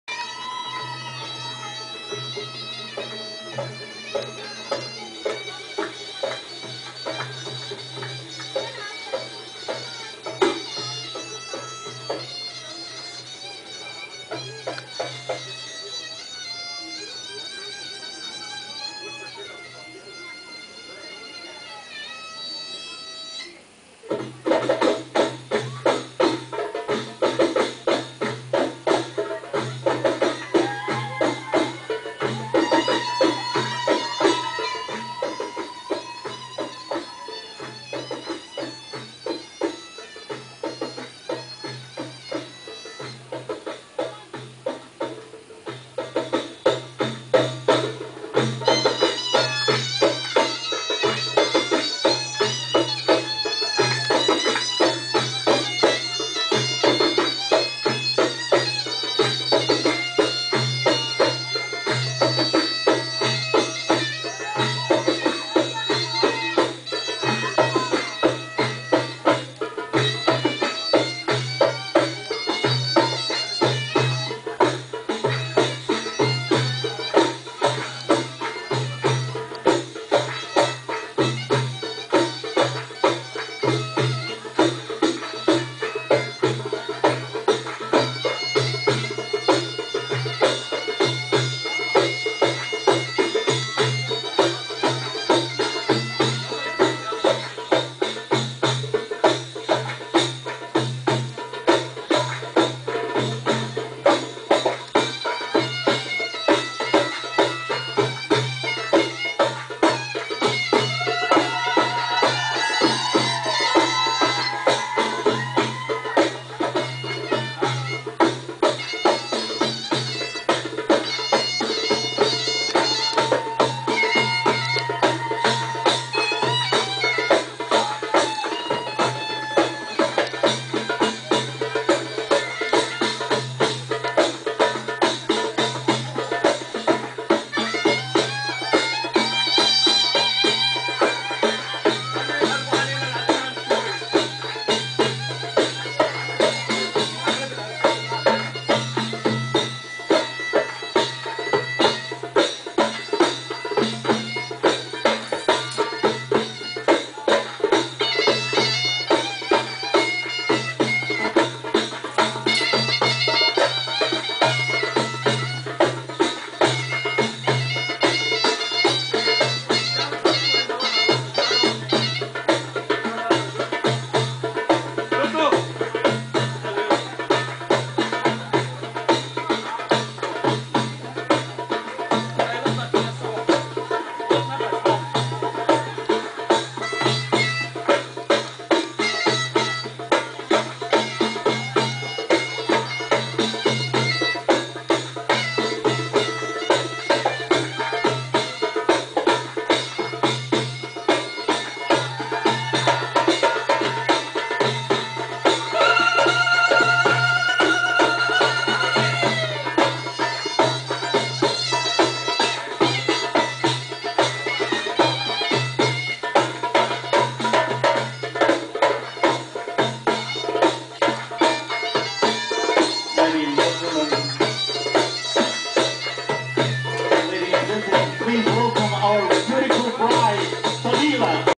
George Haddad Zaffa
George Haddad and his zaffa group performing live at an arab wedding in Washington DC.